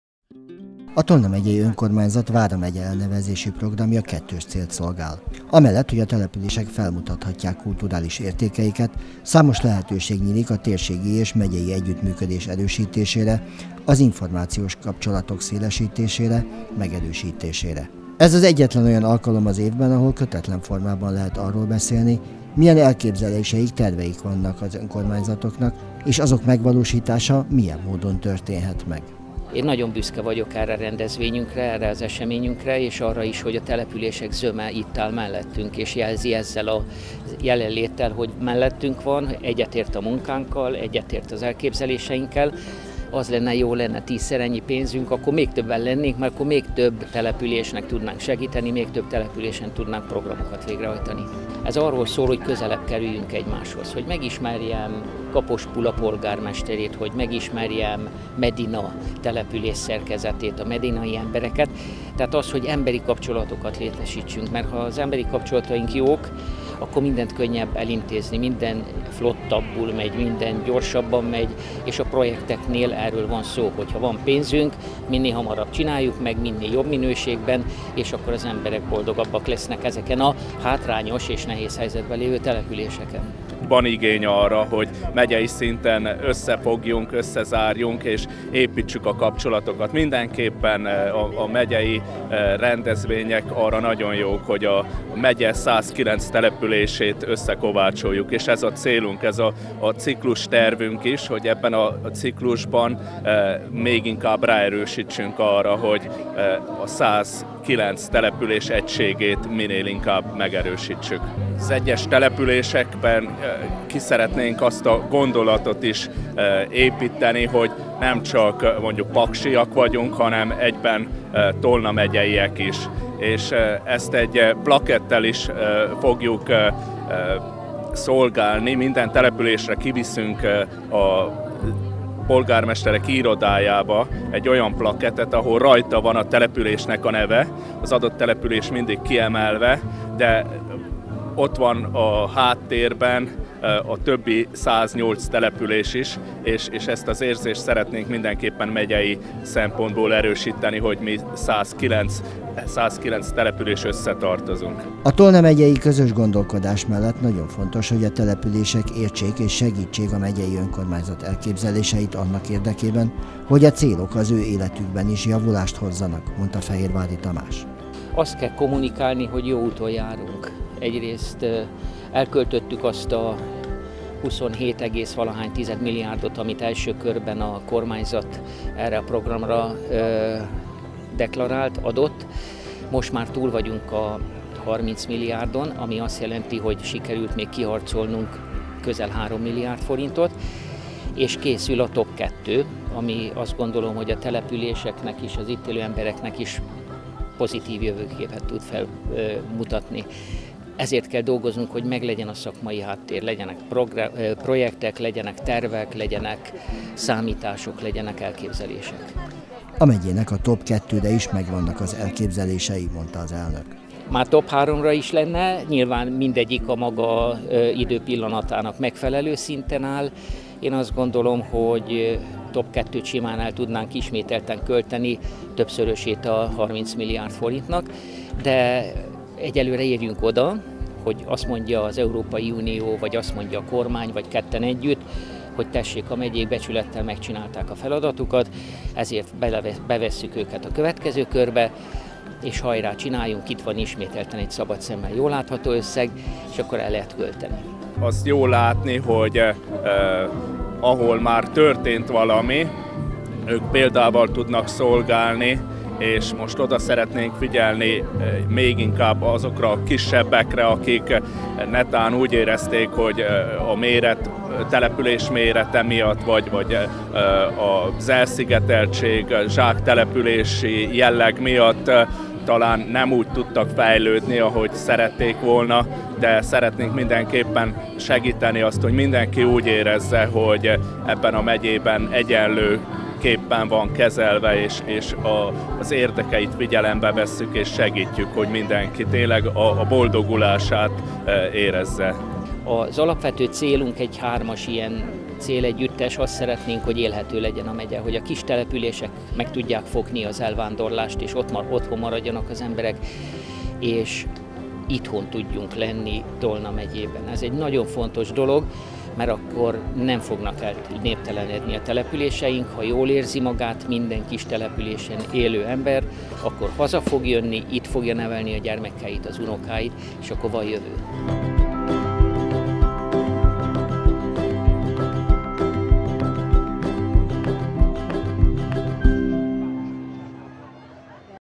Bemutatkozhatnak a települések - Medina 20200702
A tolna megyei önkormányzat minden esztendőben rendezvényt szervez, melyen bemutatkozhatnak a települések, beszélhetnek elképzeléseikről egyeztethetik terveiket, elképzelésieket.